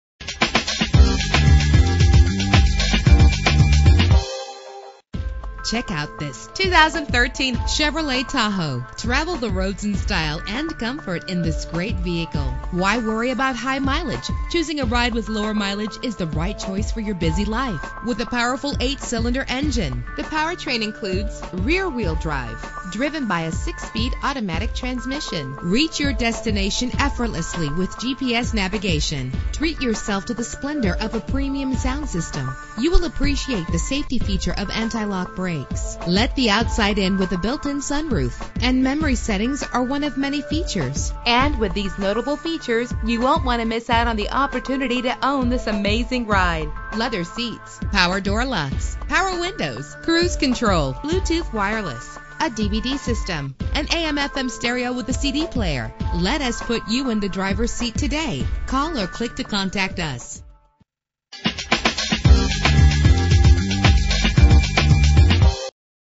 2013 Chevrolet Tahoe - Stafford TX
Year: 2013
Make: Chevrolet
Model: Tahoe
Trim: LTZ
Engine: 5.3 liter 8 cylinder 16 valve
Transmission: 6-Speed Automatic
Color: Black
Mileage: 4073
Address:
12053 Southwest Freeway